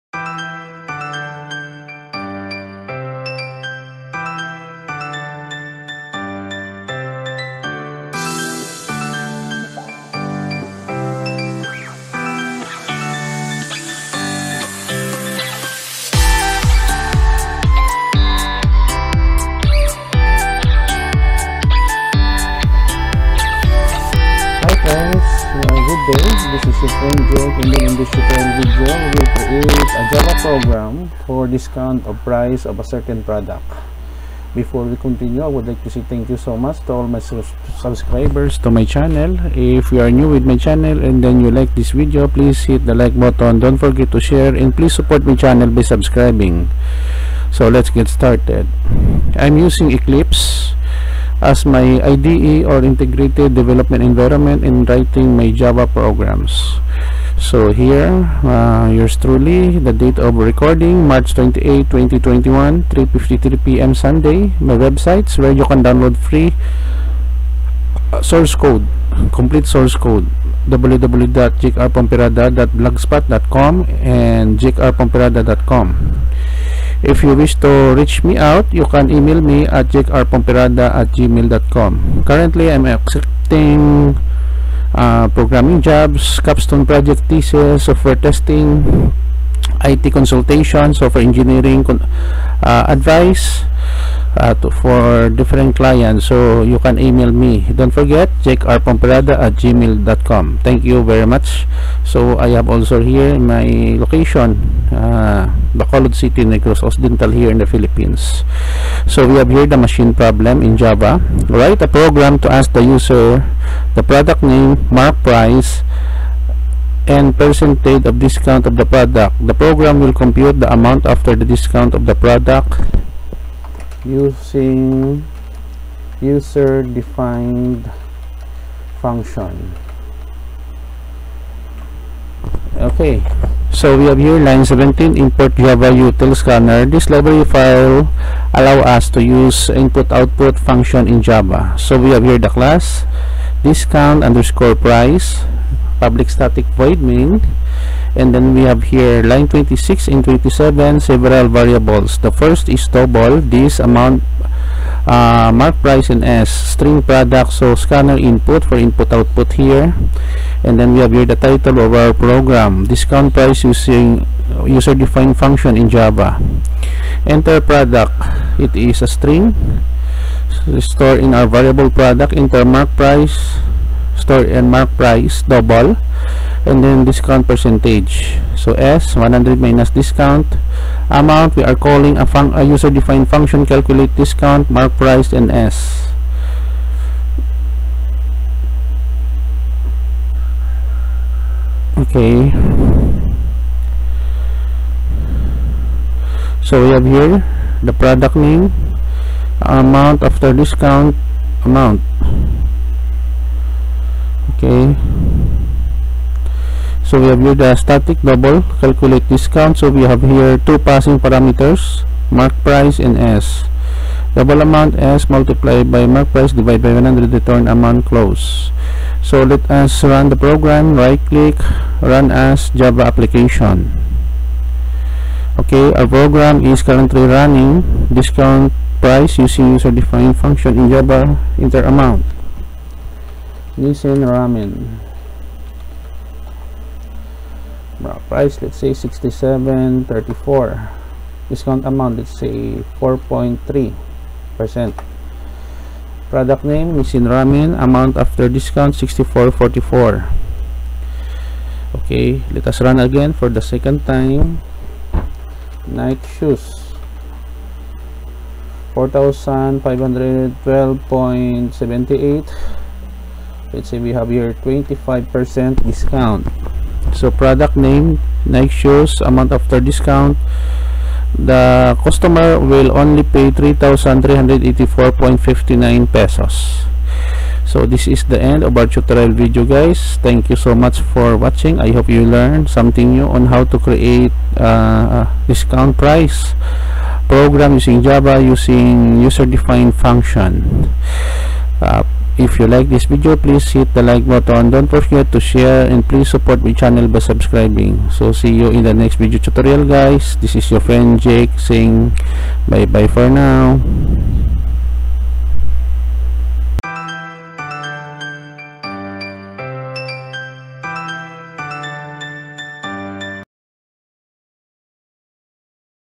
Discount Price Using User Defined Function in Java
Machine Problem

Write a program to ask the user the product name, marked price, and percent of discount of the product. The program will compute the amount after the discount of the product.